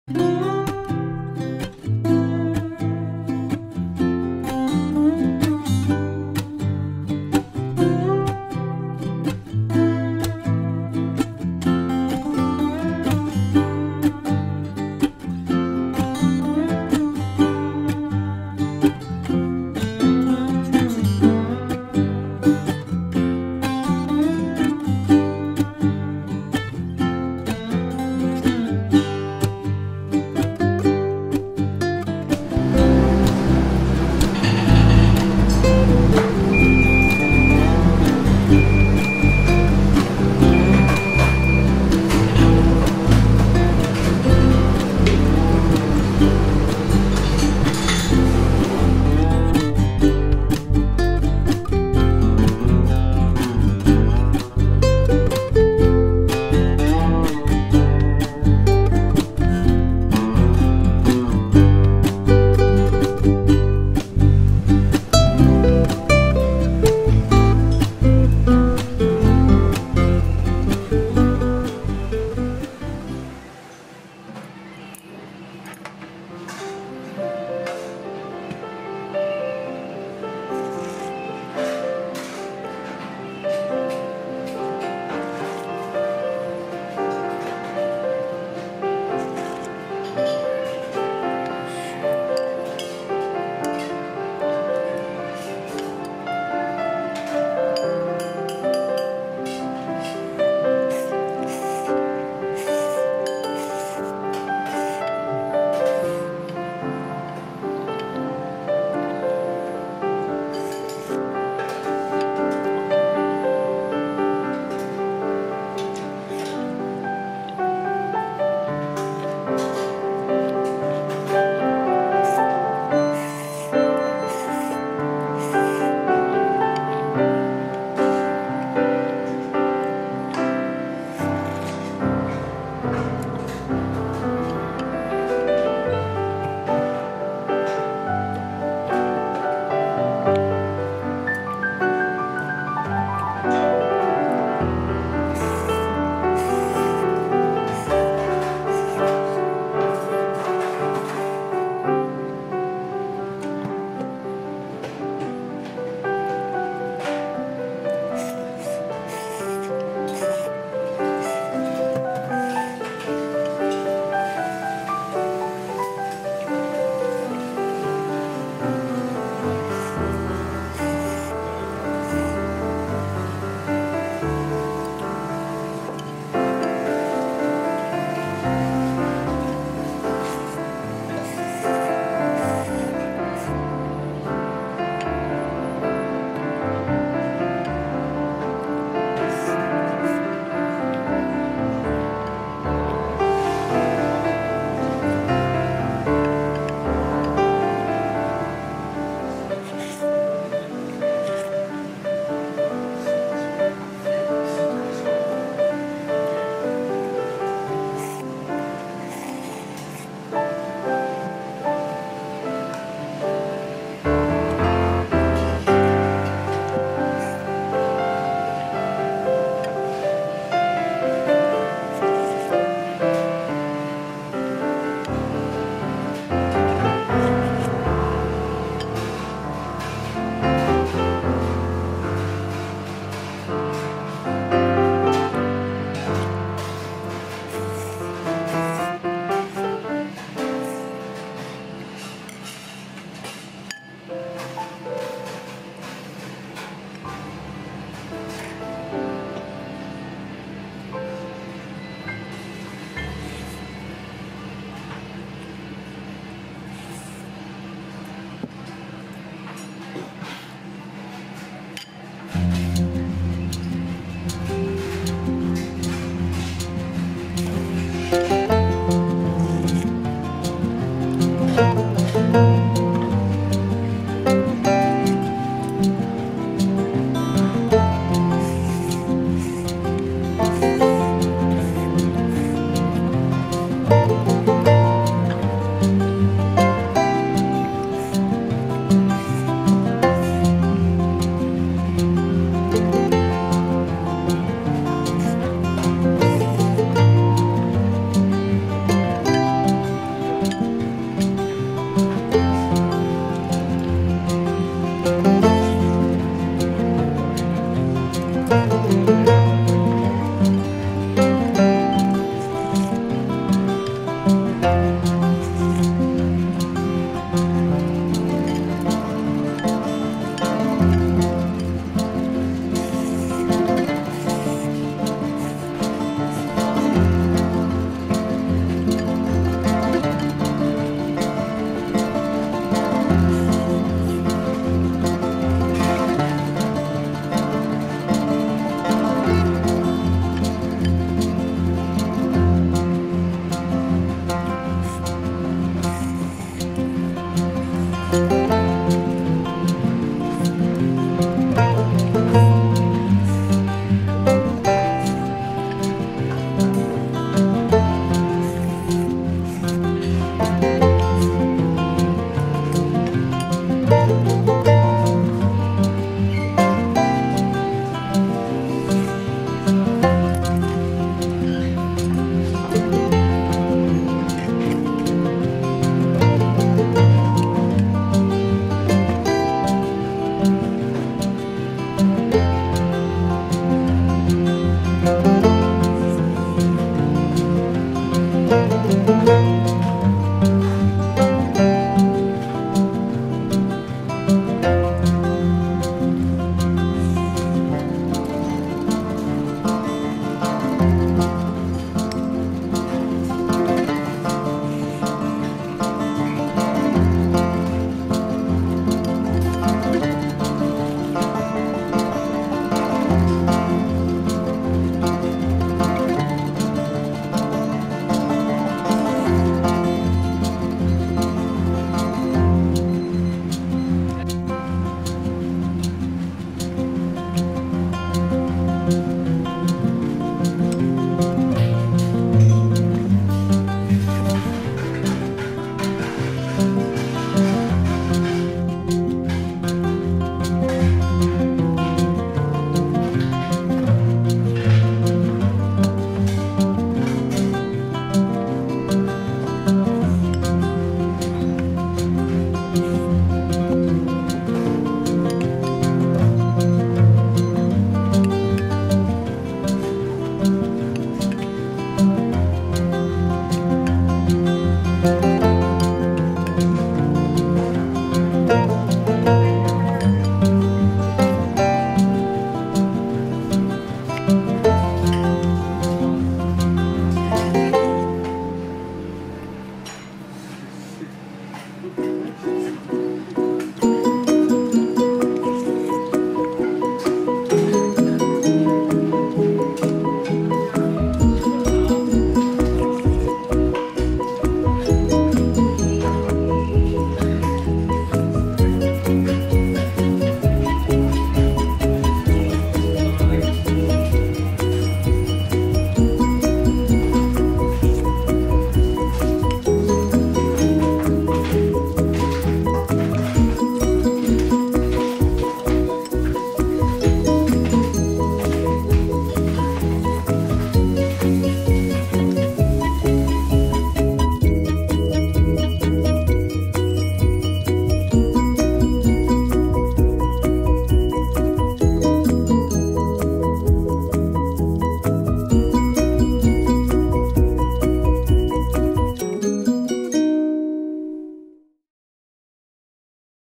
【めん結び 光の葉】My Best 5 ③ 鶏白湯らーめんの好きなお店紹介 ＃987
2018年7月に初訪問して、丁寧な麺の作りとお店のインテリアに感銘を受けました。音楽を変え再編集してみました
めん結び　光の葉